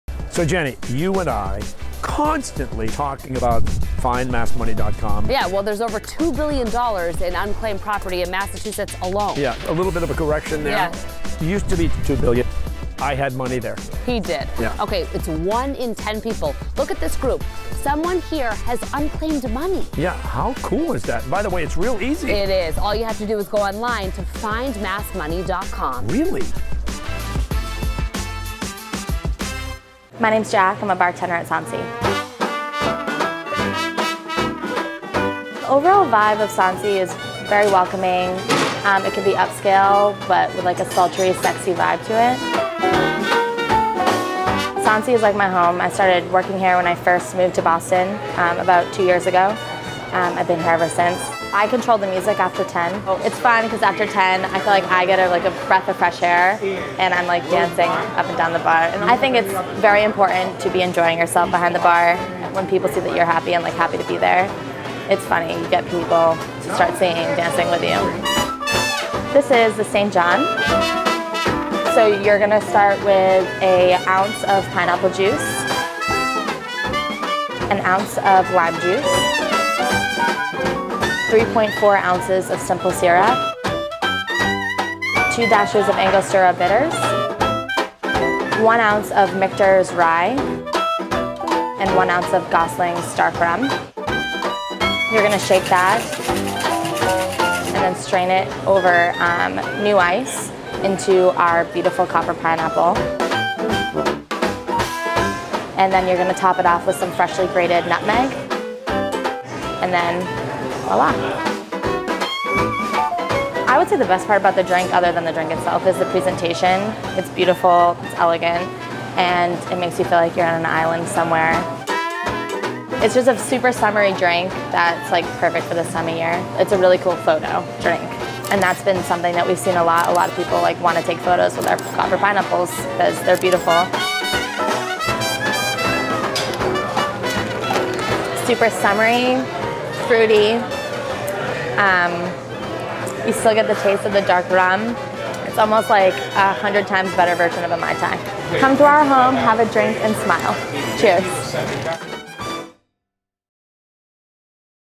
Dining Playbook: Cheers to That: Sonsie
We're chatting with a bartender from the classy Boston restaurant, Sonsie.